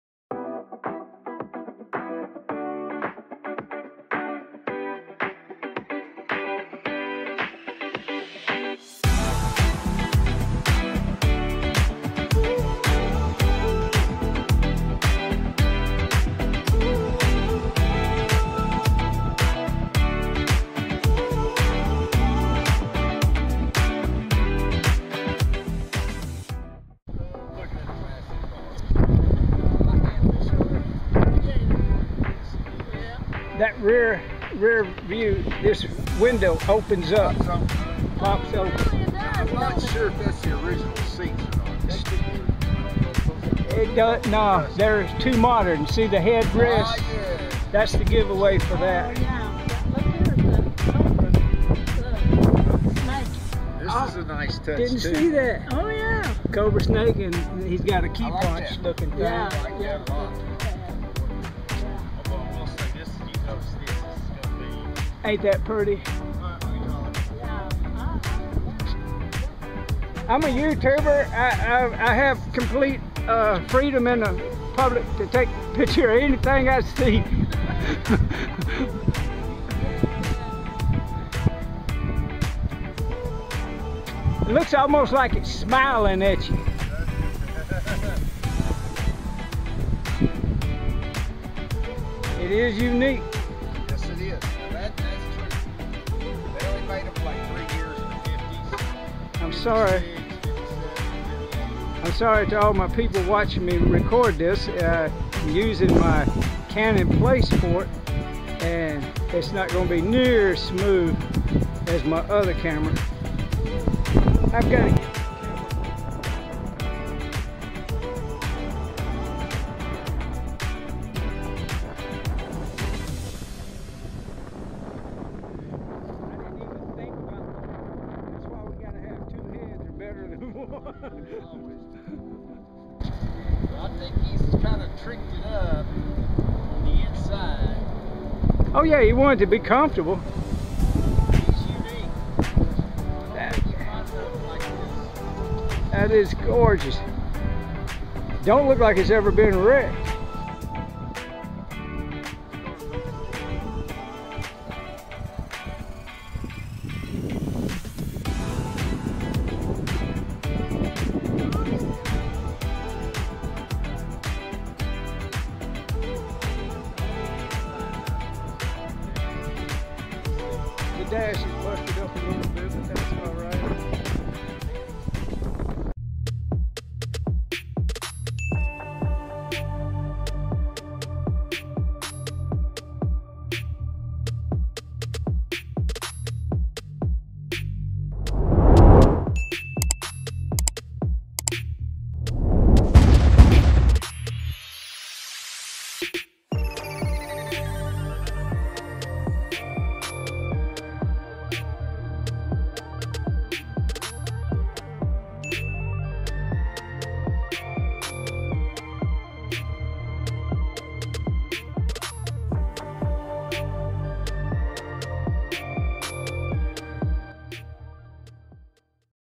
1955 Studebaker President Speedster: A Classic Beauty in Ozark MO
1955 Studebaker President Speedster: A Classic Beauty in Ozark MO
"Join Buddy Huggins as he explores the stunning 1955 Studebaker President Speedster. This rare classic car features a sleek two-tone paint job, a powerful V8 engine, and luxurious interior details. Discover the history and charm of this hidden gem and see why it's a favorite among car enthusiasts."

~ Buddha  : )